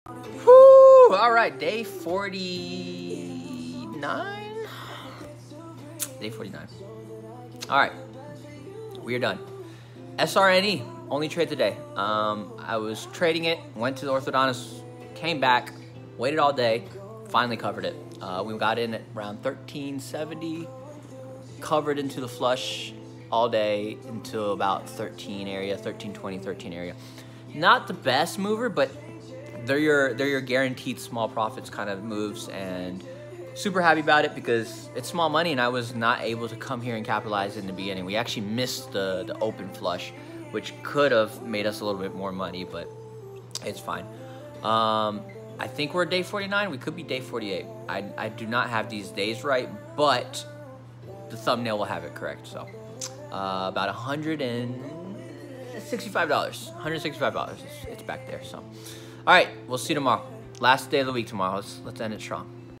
4 Day Green Day Week. Lets get one more | DAY 49 | +$165 | $4,000 to $100k Trading Challenge!
We're coming close to closing this week. One more swing tomorrow. Lets see if we hit big.

Day 49
Bankroll:
-$1,399